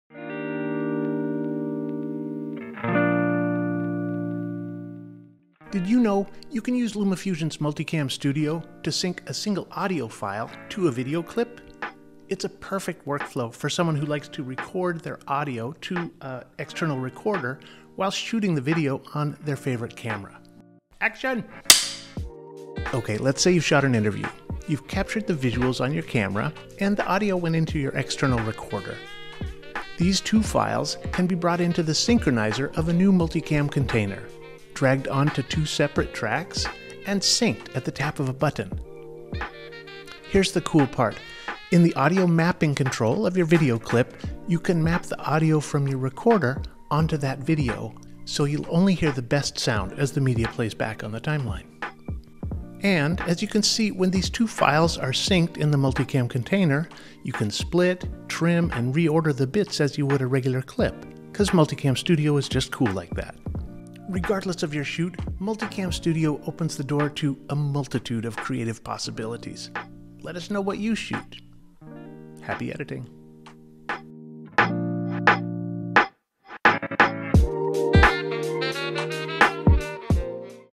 Use Multicam to Sync Audio and Video Clips
Save time in Multicam!
So you’ve shot your movie with a camera and an external audio recorder - now what? Here’s Scott to teach you how to sync these files together using Multicam Studio in just a few taps!